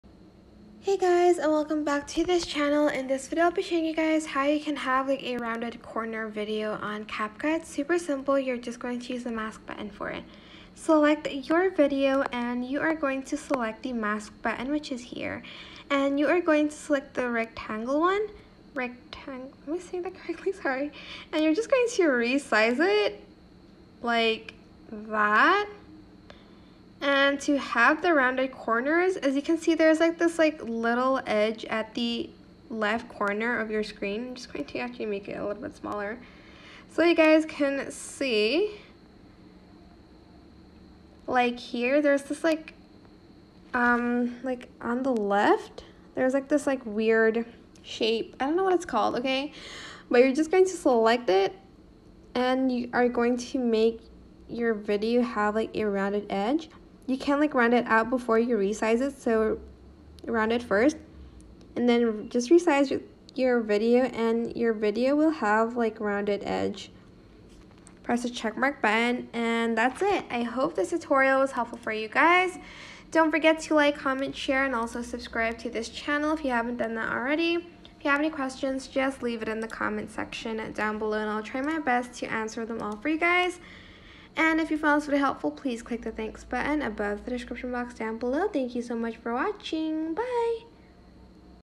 Masking On CapCut! How To Have A Rounded Corner Video On CapCut? NEW UPDATE APRIL 2023
Have a rounded corner video on CapCut by watching this tutorial.